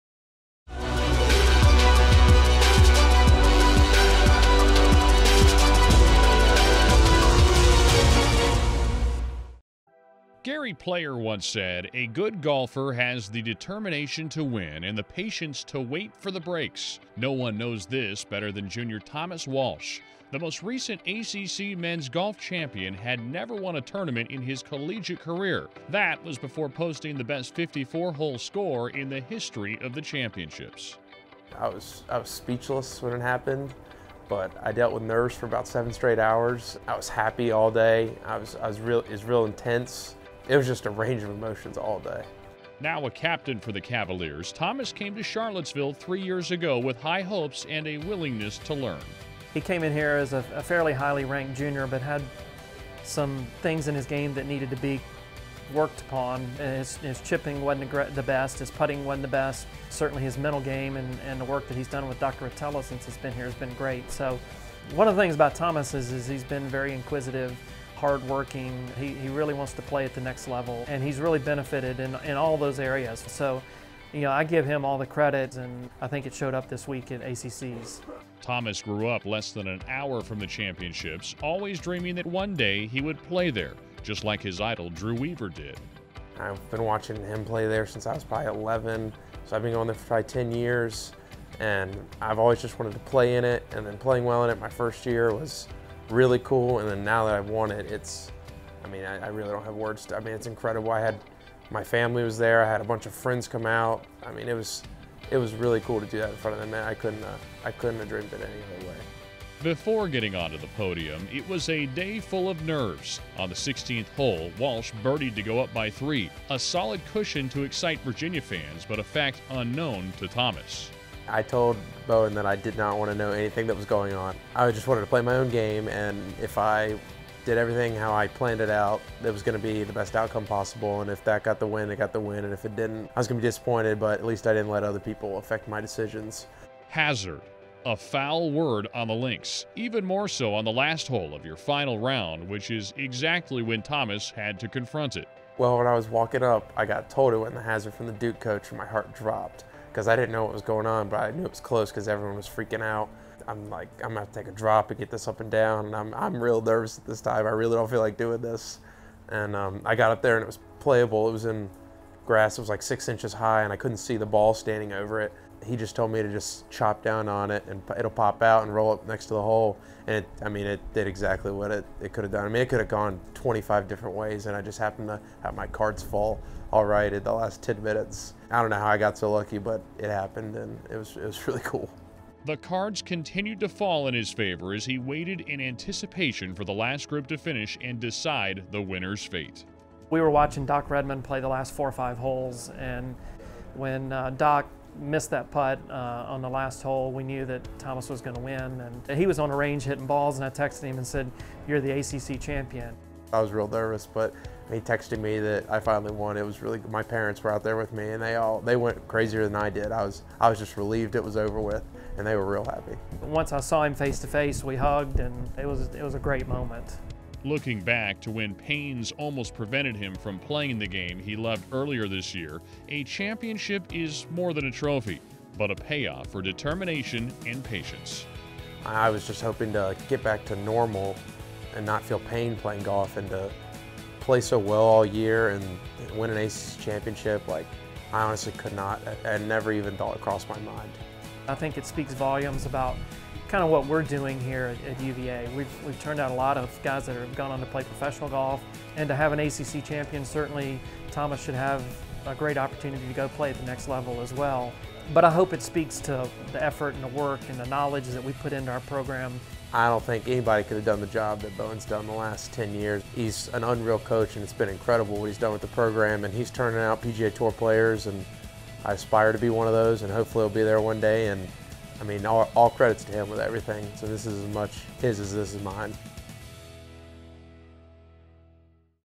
UNCOMPROMISED EXCELLENCE: Men's Golf - Thomas Walsh
The captain of the Cavaliers is the 2018 ACC Men's Golf Champion.